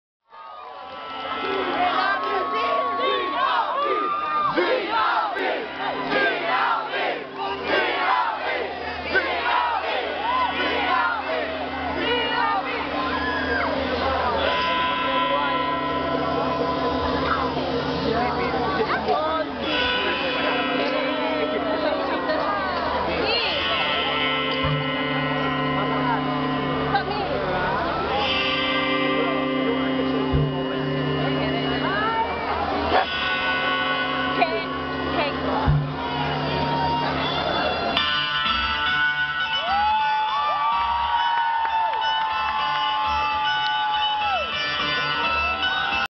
RWMF2012: Zee Avi (intro)
The intro to Zee Avi's performance at the 15th Rainforest World Music Festival in Kuching (13 July 2012)